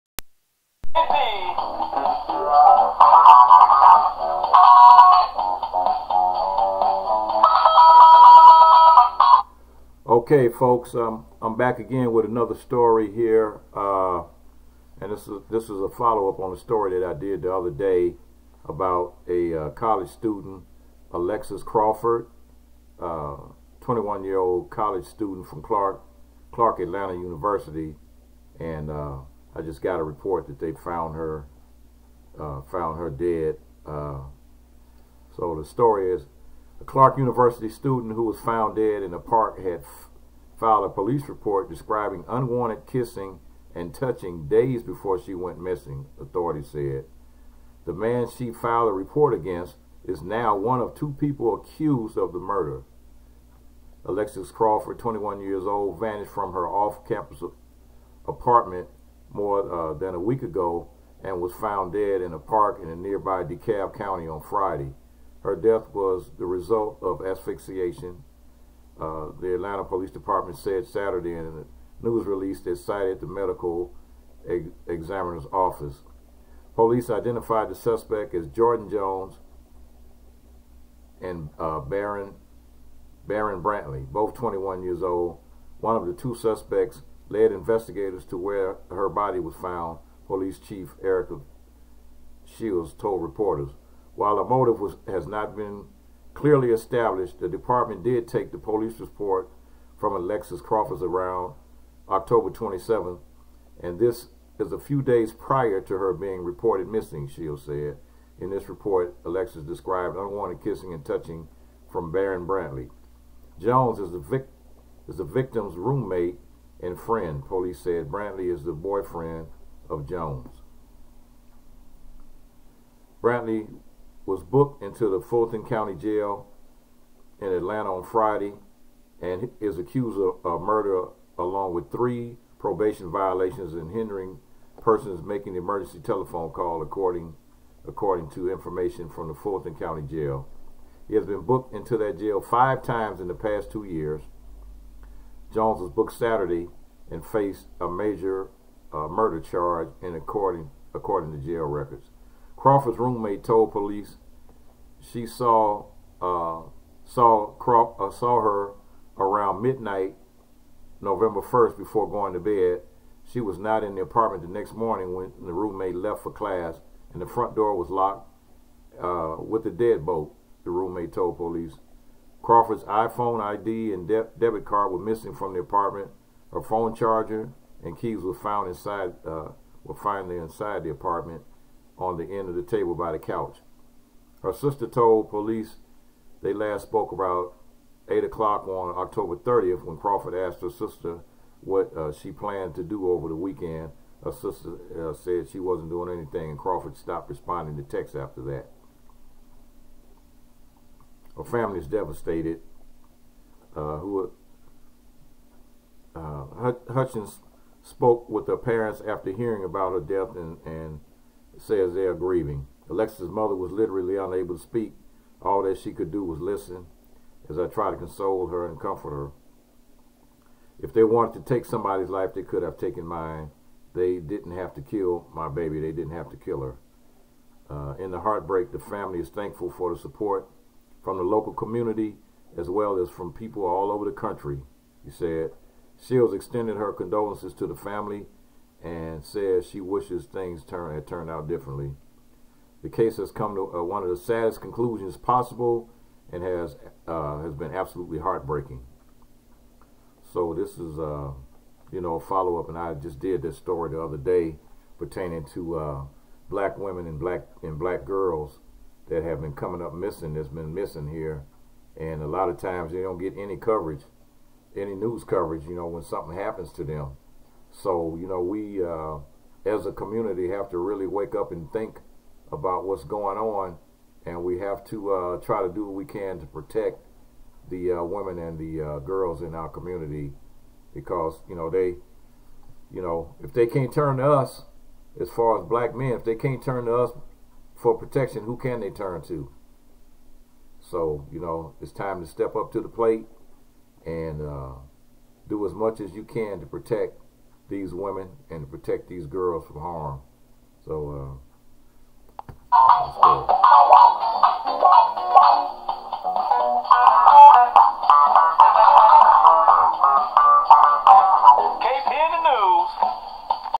video # 96 ALEXIS CRAWFORD - FOUND DEAD
ALEXIS CRAFORD - CLARK ATLANTIC COLLEGE STUDENT - FOUND DEAD. …. SUSPECT UNDER ARREST